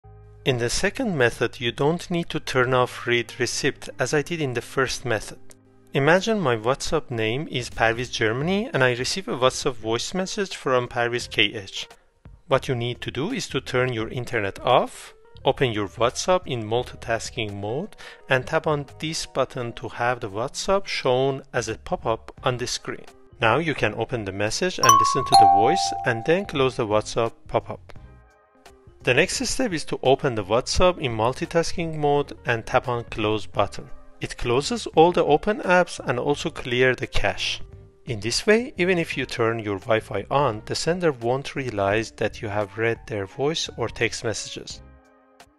How To Listen to WhatsApp Voice Message Without Sender Knowing
Ever wanted to read or listen to a WhatsApp message without the sender knowing? Sometimes, it's not about hiding but about the freedom to respond when you're ready. In today's video, I'll unveil two effective methods that let you sneak a peek into your messages without leaving a trace.

Method 1: This is a straightforward method right within WhatsApp but with a minor catch. By diving into the app settings, you can disable “read receipts” to stop the blue ticks that indicate a read message. But remember, this also means you won't know when someone else reads your messages. And, for group chats? This trick won't work.

Method 2: Now, this is my exclusive method you won't find anywhere else. Without disabling “read receipts”, I've found a clever workaround to access your voice and text messages without the sender being any wiser. With some multitasking and quick steps, you'll be able to read or listen to messages without the fear of that blue tick. Curious? Stick around till the end.

Throughout this tutorial, I'll be using demo chats between 'Parviz Germany' and 'Parvizkh' to give you a visual step-by-step guide. So if you're someone who values a bit of privacy or maybe just wants to avoid that momentary anxiety of having to reply immediately, this video is for you.